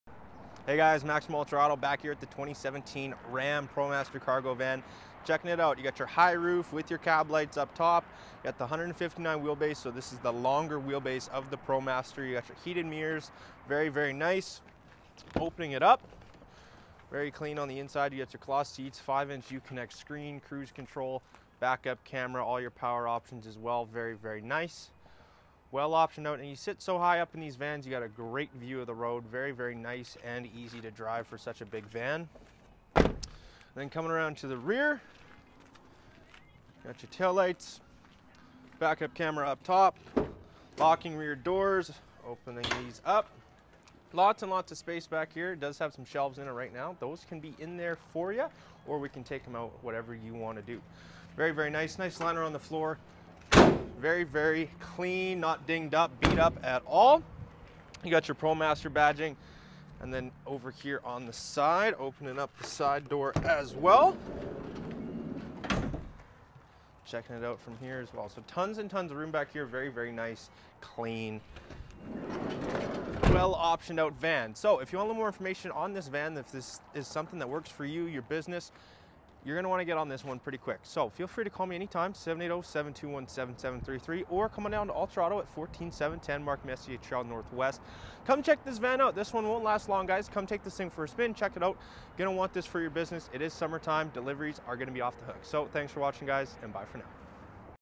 2017 Ram ProMaster Cargo Van 2500 | Stock M0721 | UltraAuto
Check out this 2017 Ram ProMaster Cargo Van 2500 available here at Ultra Auto.

CLEAN CARFAX | High Roof | 159" WB | 420.0 Cu.Ft | 5100 LB Tow | Voice Command | Backup Cam | Van 6-Speed Automatic w/OD

Stock

Ultra Auto
14710 Mark Messier Trail NW
Edmonton, AB T6V 1H5
Canada